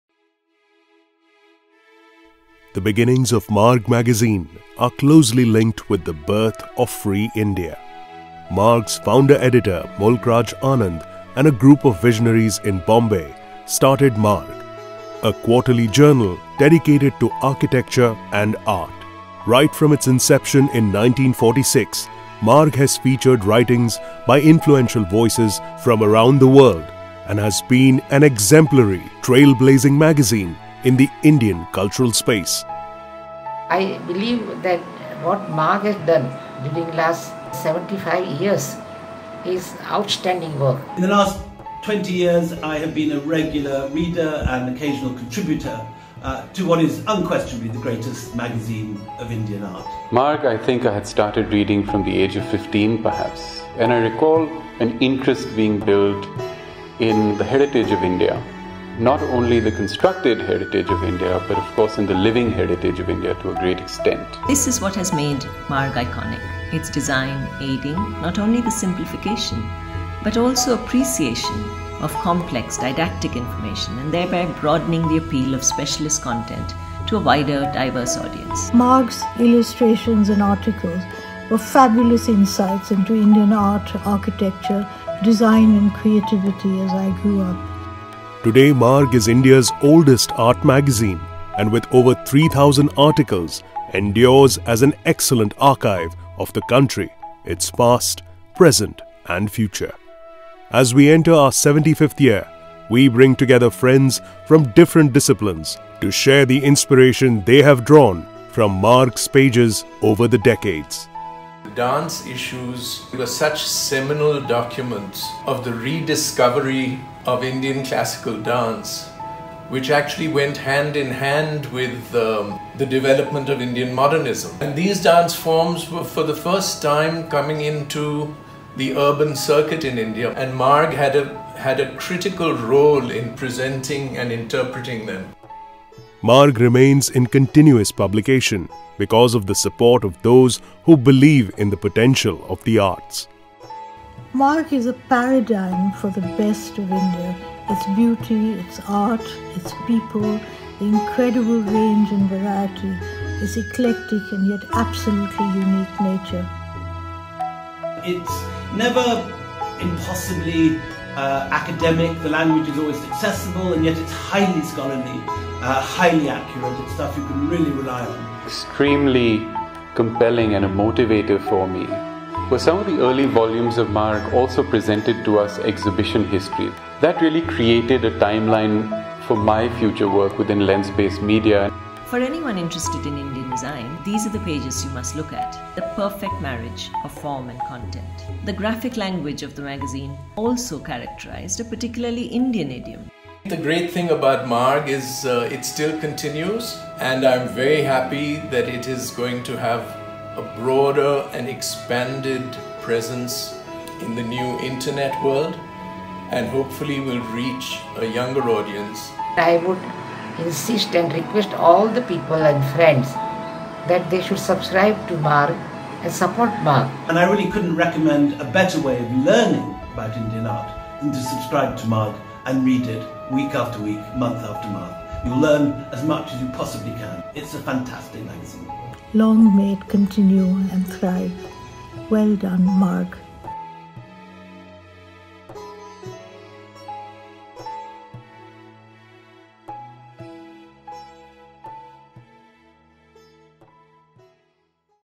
Marg | Why an arts magazine today?
Leading cultural thinkers on how Marg magazine has shaped their perspectives. Our 75-year archives needs your support to survive. We seek your participation to continue.